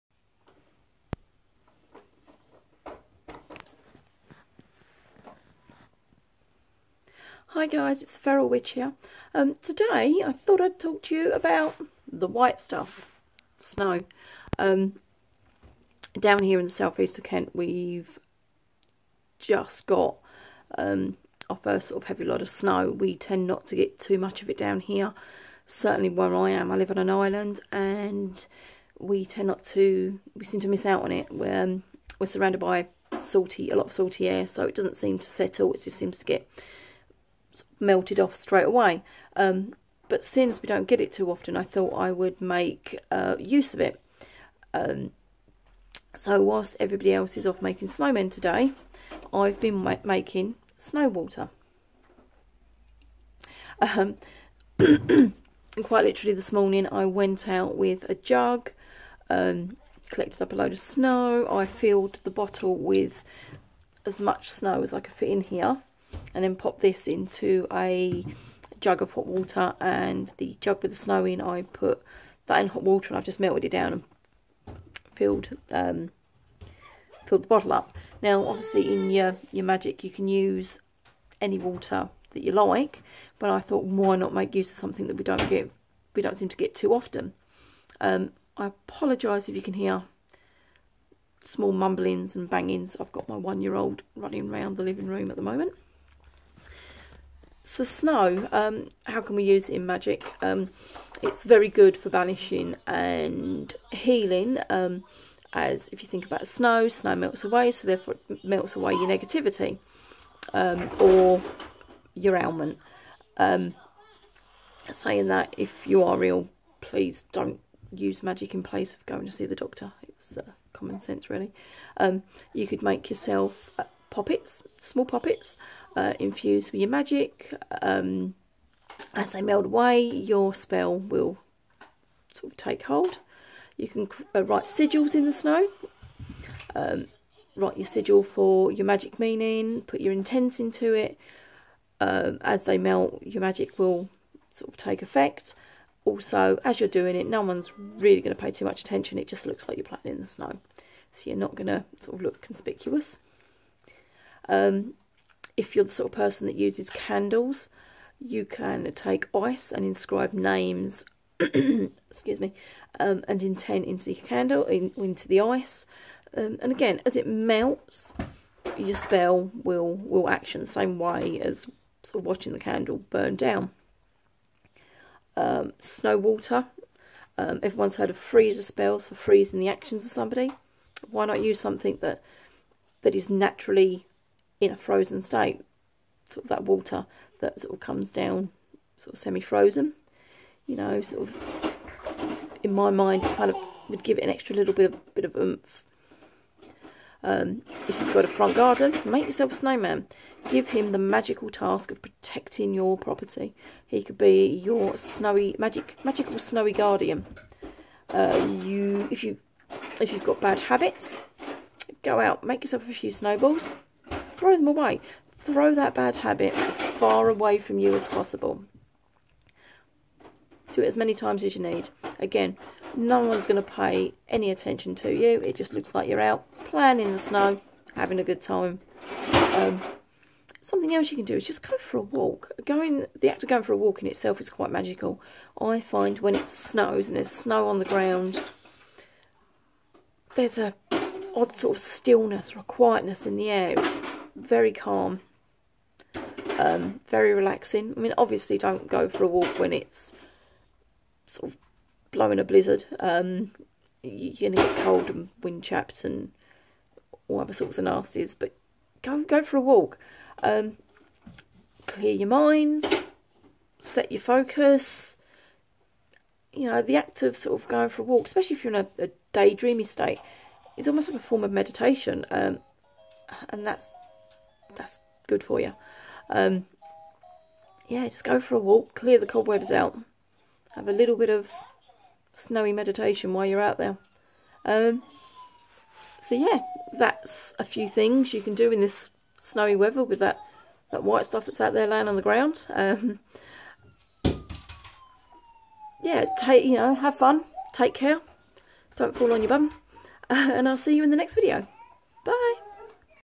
Lets Talk Snow
A quick chat about snow water and how to use the current UK weather in your magic.